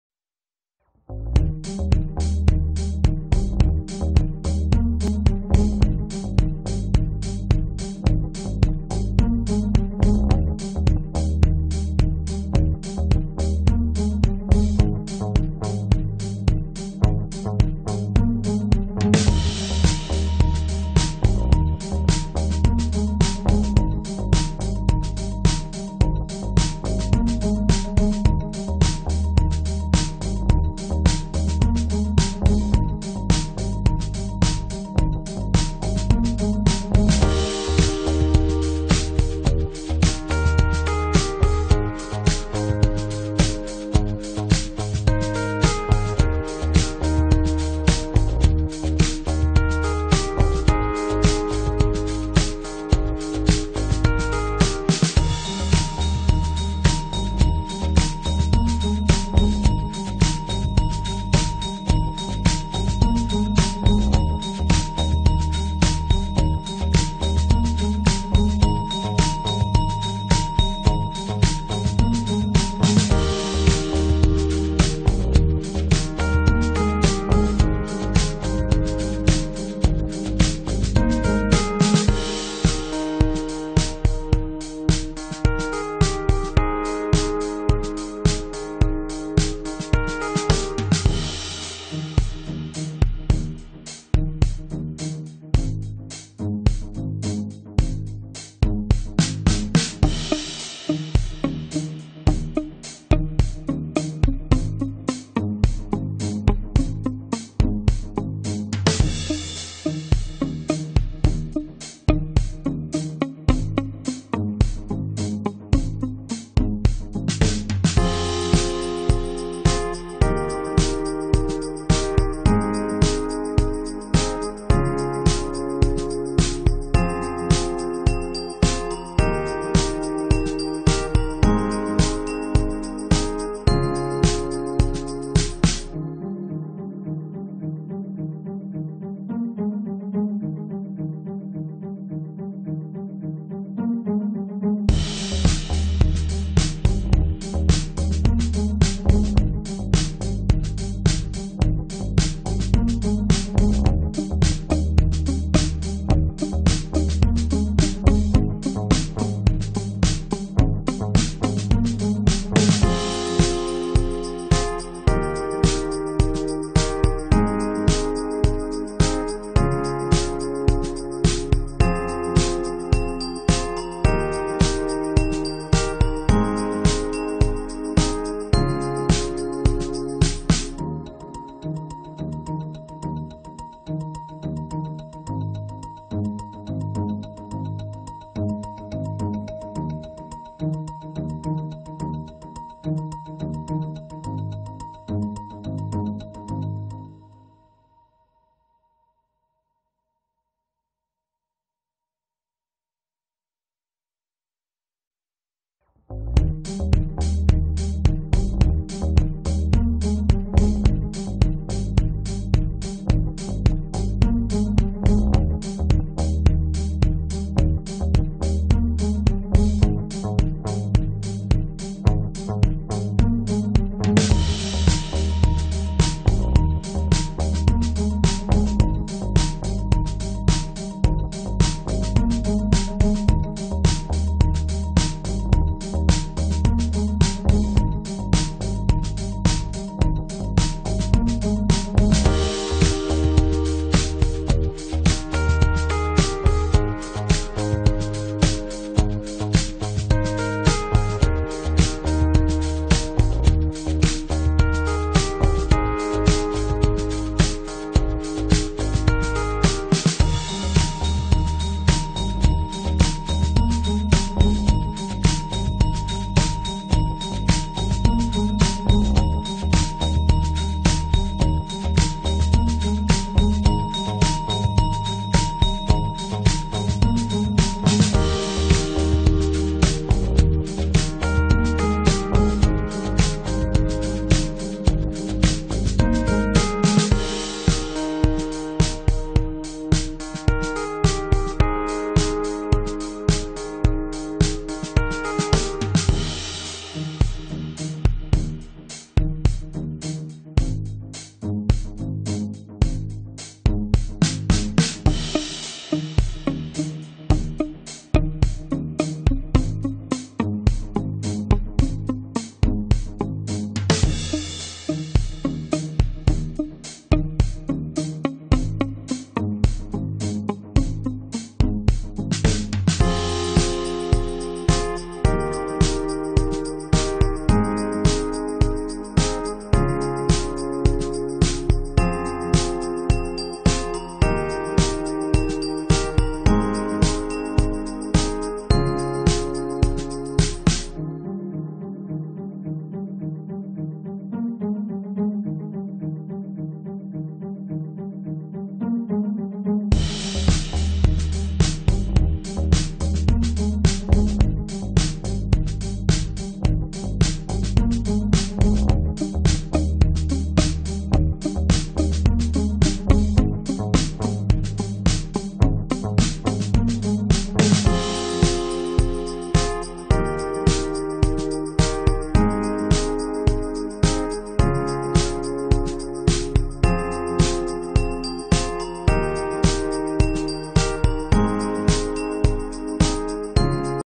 lifetravel TUNISIA 6 ● Capbon peninsula ●
The Capbon Peninsula, which has sprung out into the Mediterranean under Tunis, is rich in oranges due to the mild weather.